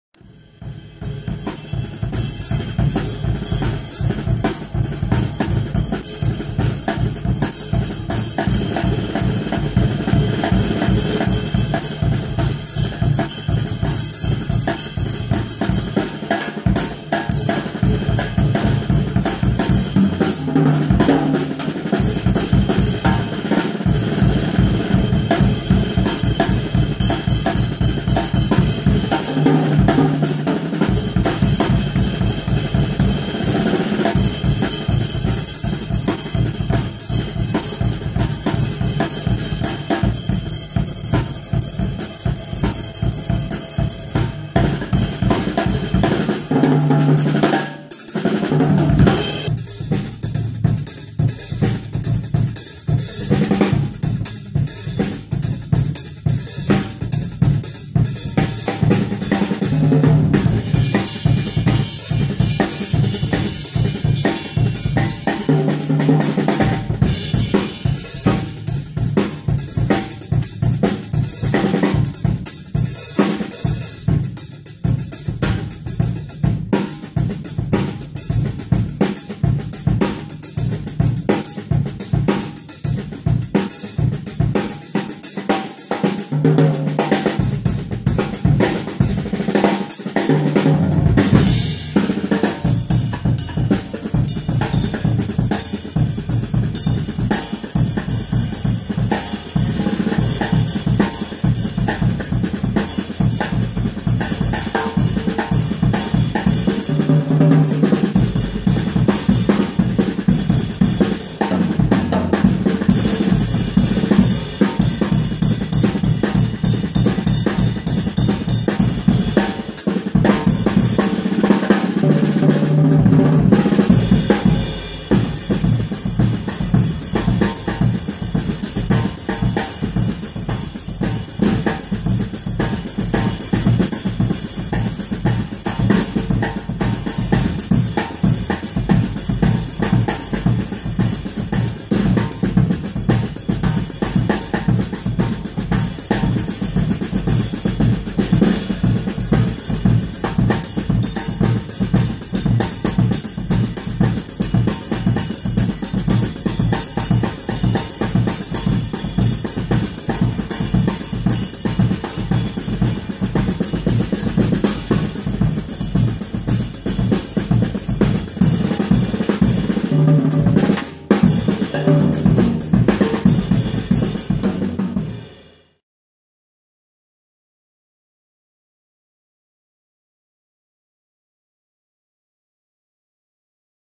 Cesar Banana Durañona en Mj Music
Recibimos en nuestras oficinas a Cesar Banana Durañona. Testeo de nuestros productos y armado de su nuevo set Sonor, Sabian, Evans y Promark.